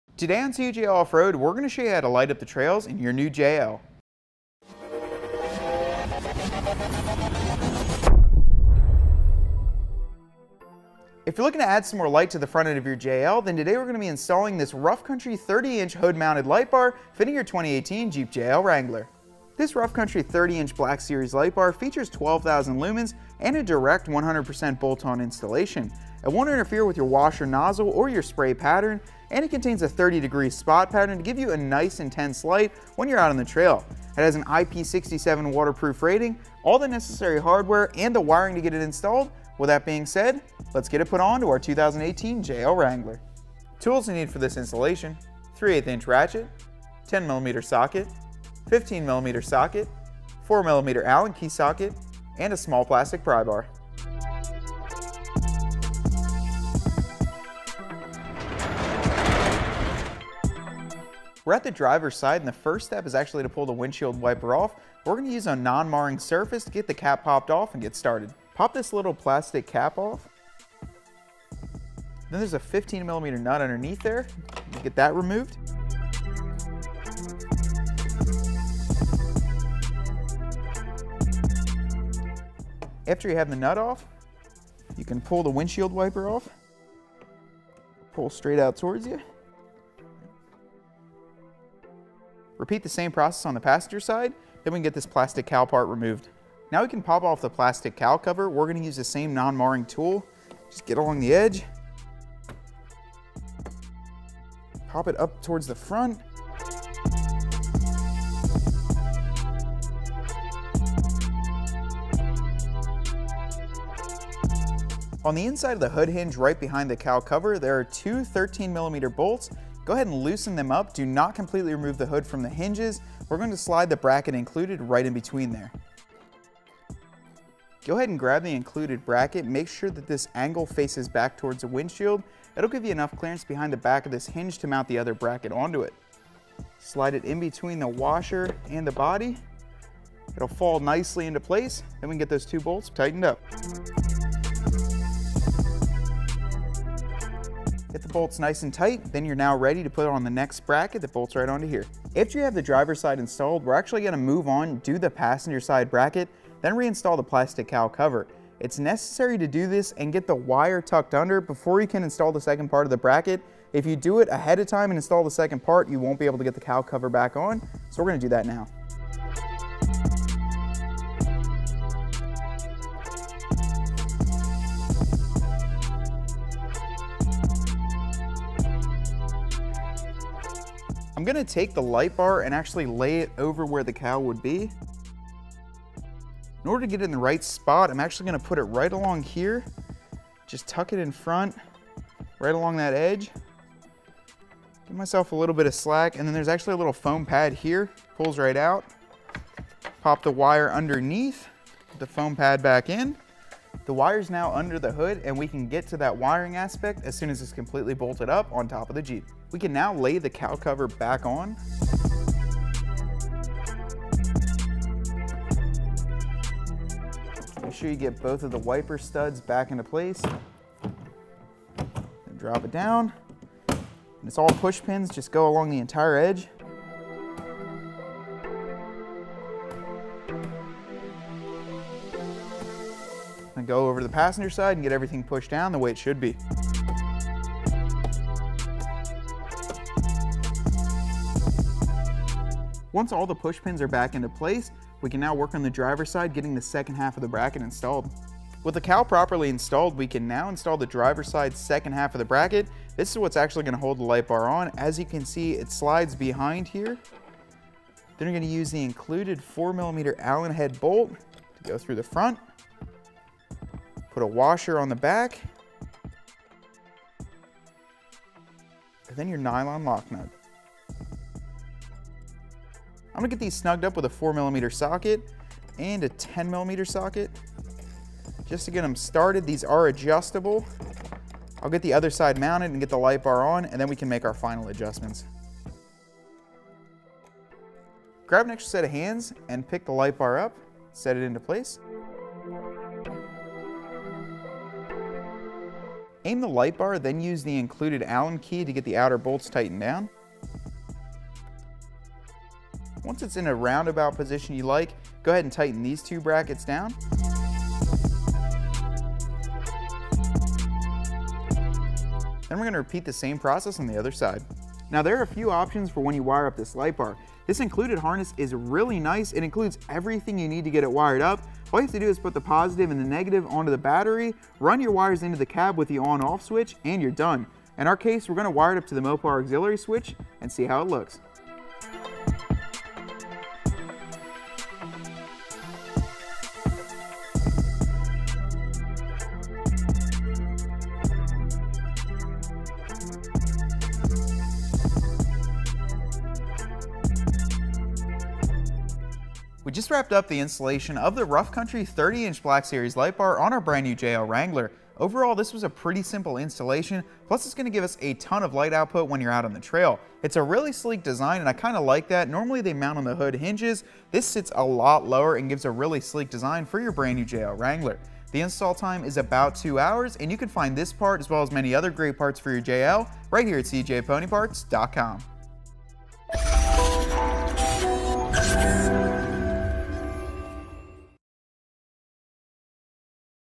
2018-2020 Jeep Wrangler JL: Rough Country 30" LED Hood Light Bar Kit Install
Matt shows you how to install a Rough Country Black Series 30" LED Hood Light Bar Kit on your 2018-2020 Jeep JL Wrangler.

When you take your rig out at night, lighting up the trail is very important, so give your off-road adventure the help it needs with this awesome 30-inch LED light bar from Rough Country.

Featuring 12,000 lumens, 150 watts, a 30 degree spot beam, thirty 5-watt Cree LEDs, an IPt67 waterproof rating, long-lasting die-cast aluminum housing, and more.

Acting as a 100-percent bolt-on install, this premium LED light bar installs quick and easy.